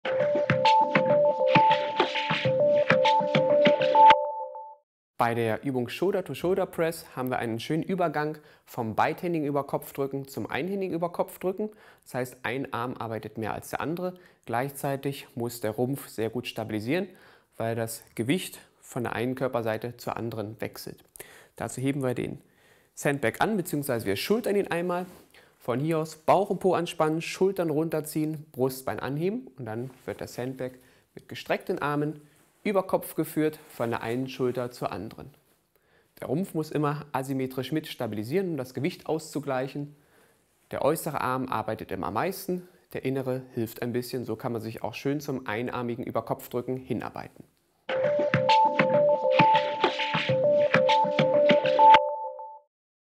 Sandbag Training mit Dr. Till Sukopp: Shoulder to Shoulder Press
Dr. Till Sukopp stellt unterschiedliche Sandbag-Übungen vor, hier: Shoulder to Shoulder Press.